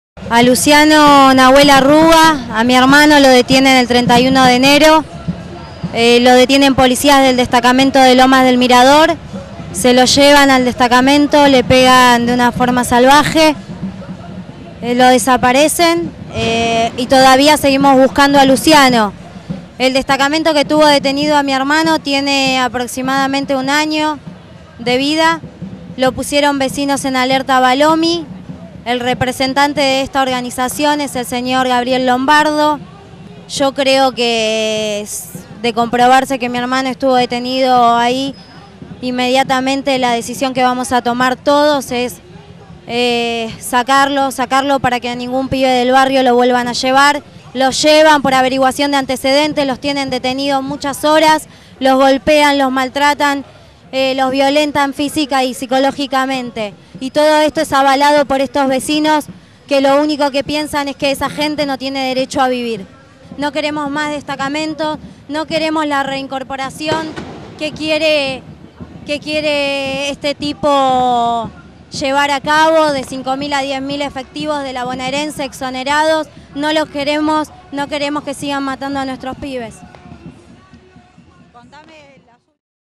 La hermana de LUCIANO ARRUGA, desaparecido por la bonaerense | informe TVPTS
Entrevista a la hermana de luciano arruga, desaparecido por la policia bonaerense de argentina el 30 de enero del 2009 | Entrevista en Plaza de mayo, Argentina. para el programa diario CONTRAPUNTO, opinon y politica desde la izquierda